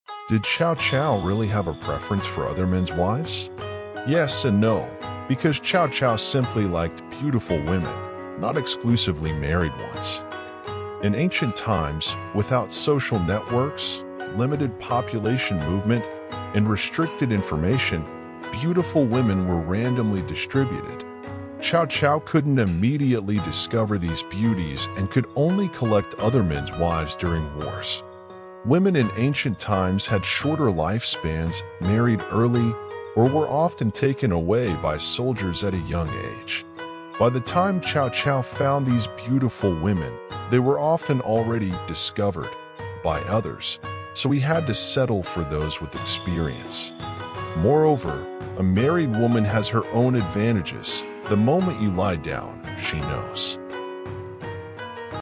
Did Cao Cao really have a preference for other men's wives?
What made Cao Cao a legend? Was he the cold-hearted tyrant as often depicted in folklore, or a visionary leader ahead of his time? In this in-depth documentary, we traverse the line between myth and history to bring you a closer look at Cao Cao's military tactics, political maneuvers, and his indelible impact on Chinese history.

🐉 Highlights of the Video:

The Rise of Cao Cao: From a minor government official to the Chancellor of the Eastern Han dynasty.
Military Mastery: Analyzing Cao Cao's landmark battles and the strategies that led to his supremacy.
Governance and Reform: How Cao Cao's policies shaped the political landscape of ancient China.
Cultural Influence: Exploring Cao Cao's contributions to poetry and the arts, contrasting his warlord image.
The Three Kingdoms Context: Understanding the era that forged and eventually was reshaped by Cao Cao.
📜 Dive into the archives of history as we unearth facts and dispel fiction, painting a portrait of Cao Cao that challenges the traditional narratives.